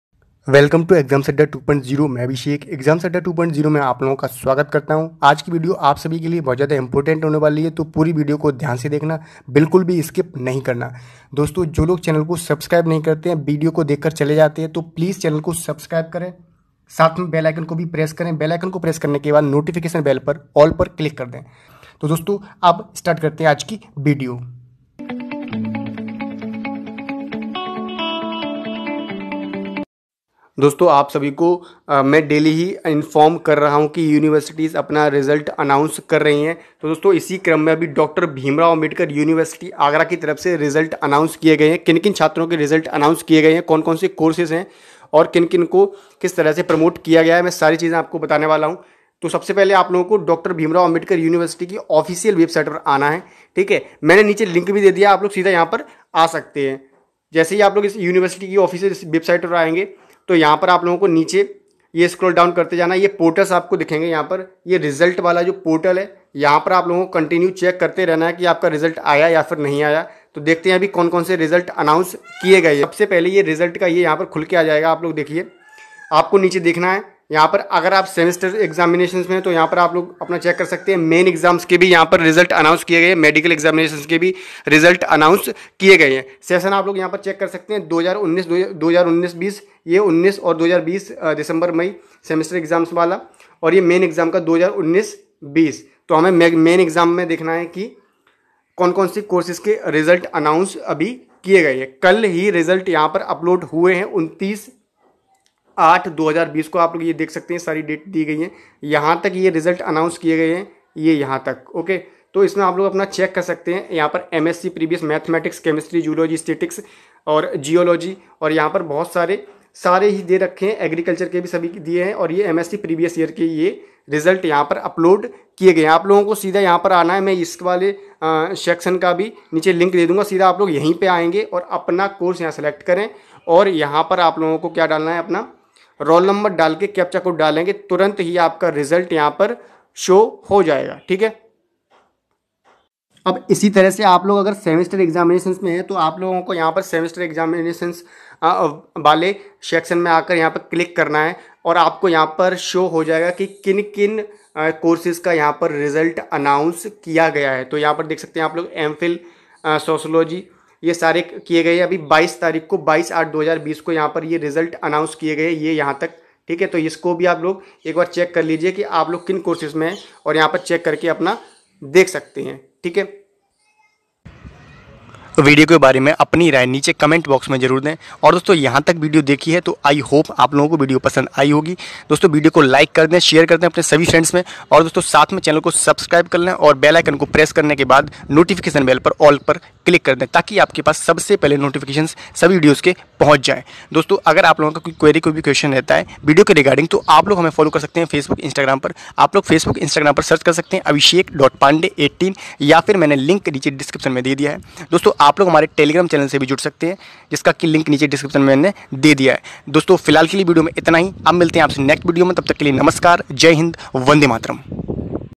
खुशखबरी : Dbrau Declared Result 2019-20 session || आगरा यूनिवर्सिटी ने भी कर दिया रिजल्ट जारी 😁🙏
In next vedio I'll try to resolve your problems.

Like karein share karein apne dosto ke sath. University ke Students hain.